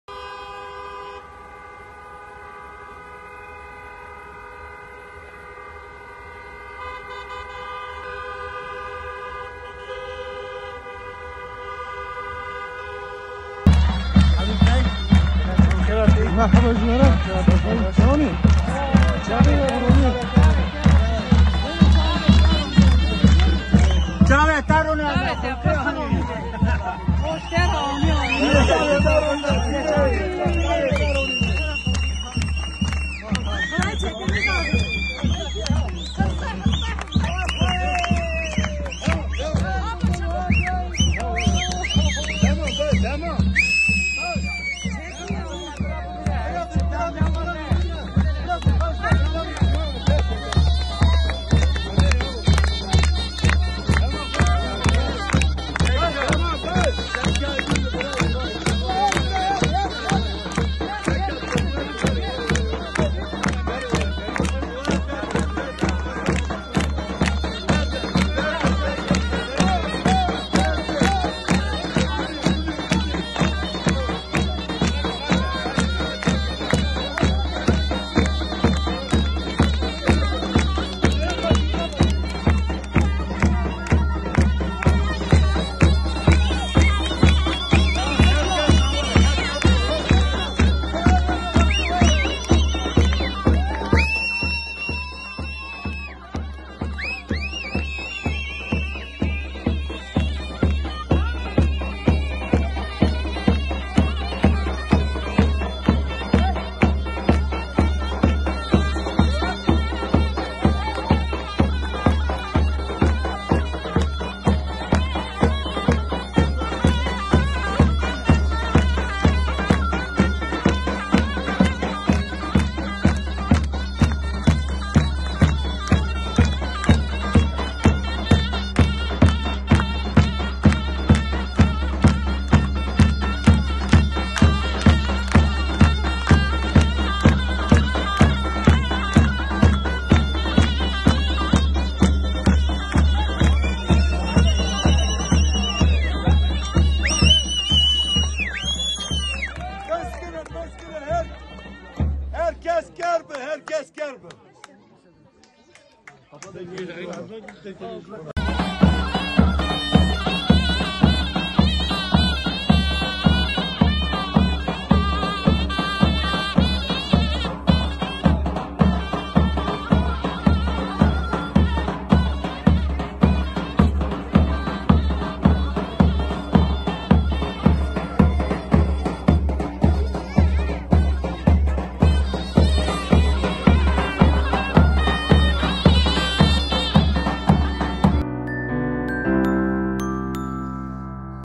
32 YILIN ARADAN TAHLİYE EDİLEN GÜNEŞ’E KİTLESEL KARŞILAMA
Şakran Cezaevi’nden 32 yılın aradan tahliye edilen Hulki Güneş memleketi Gimgim’de kitlesel bir şekilde karşılandı.